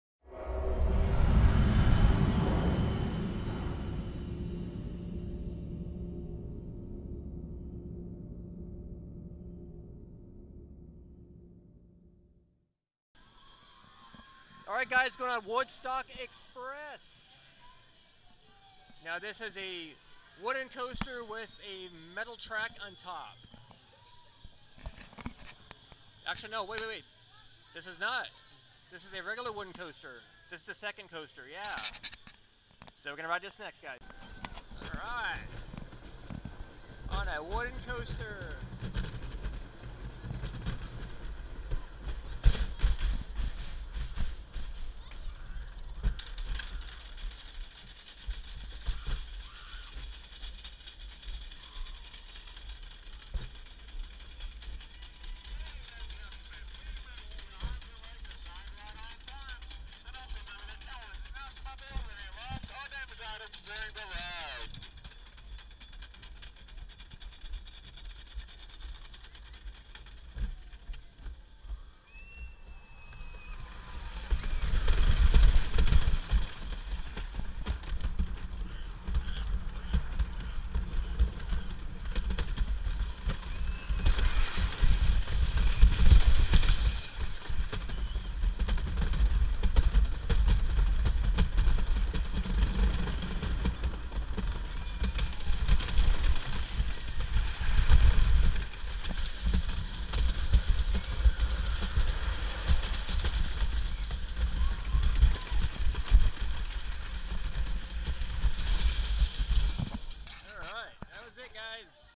Woodstock Express Wooden Coaster at Carowinds
My ride on Woodstock Express at Caowinds Theme Park.